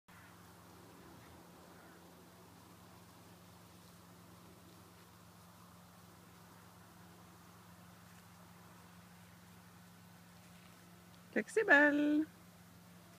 Flexibelle expecting puppies in a week.
Our lovely 3 year old Golden retriever Flexibelle du Bois de la Rayere expecting puppies in about a week.